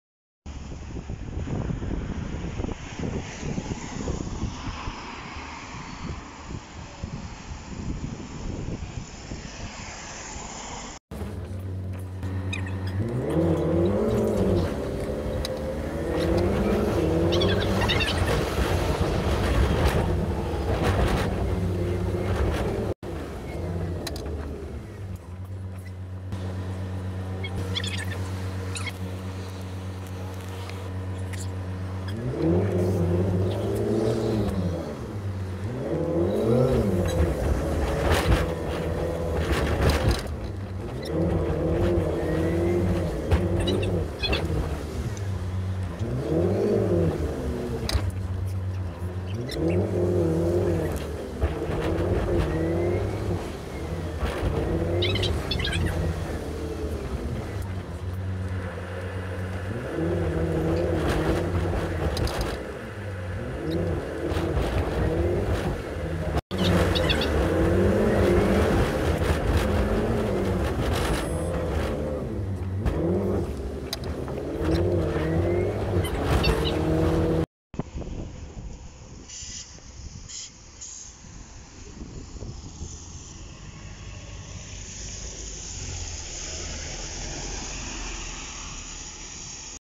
Route Visual: Go Ahead Ride: Wright Gemini 3 B5TL 11521 (152-D-17790), Route: 63 Towards: Kilternan
Plis like and sub!! This was a slow ride from York Road to Monkstown Avenue. I went with my friend @DublinBusSpotter.

Bus and Route info:
Operator: Go Ahead
Route: 63
From: Dun Laoghaire
Towards: Kilternan
Via: Carrickmines Retail Park
My Ride: York Road, Mounttown Road - Monkstown Avenue, Roundabout
Stops I Rode: 2046, 2047, 2048, 2049, 2050, 2051, 2052, 2053
Model: Wright Gemini 3 B5TL 115**
Fleet Number: 11521
Rego Plate: 152-D-17990
Ex or New: Ex-Dublin Bus SG136
Garage: Ballymount
Time: 12:05
Date: 29/1/2021